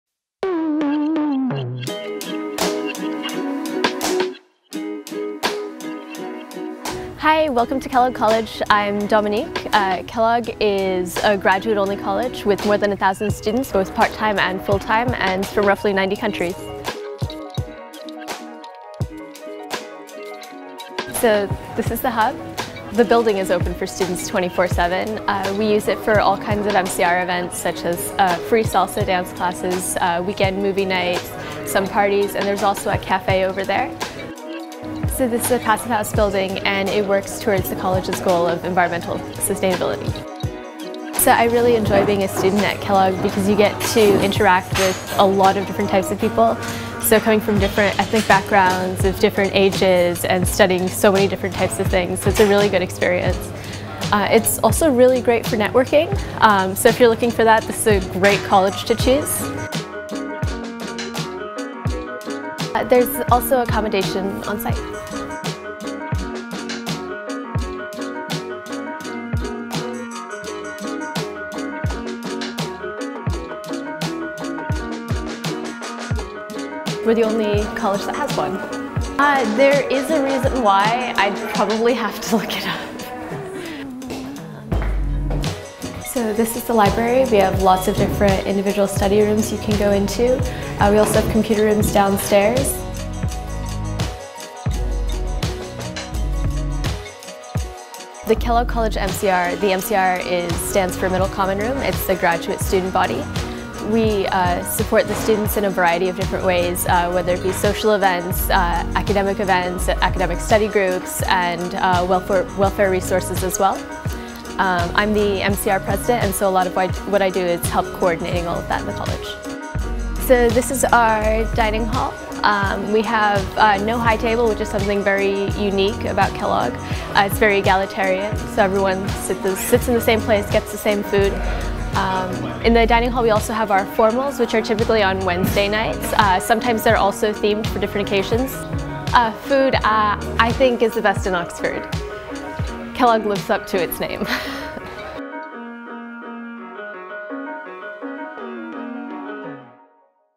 Kellogg College: A Tour
Student Dominique gives a tour of Kellogg College, a lively and diverse academic community offering a very distinctive Oxford experience.

00:00 - Welcome to Kellogg
00:18 - The Hub
00:42 - Being a student at Kellogg
01:03 - Accommodation
01:24 - Library
01:38 - The MCR
02:00 - Dining Hall